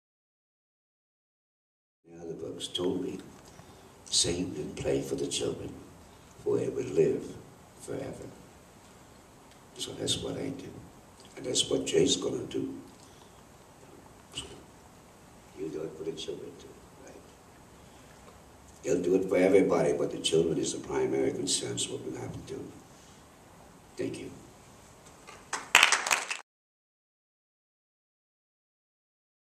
Eddie Kamae And Jake Shimabukuro (for the sake of the children).mov
Eddie Kame and Jake Shimabukuro at the Na Hoku Music Festival. Eddie talks about why he and Jake play the ukulele...it is for the sake of the children...teaching the next generation.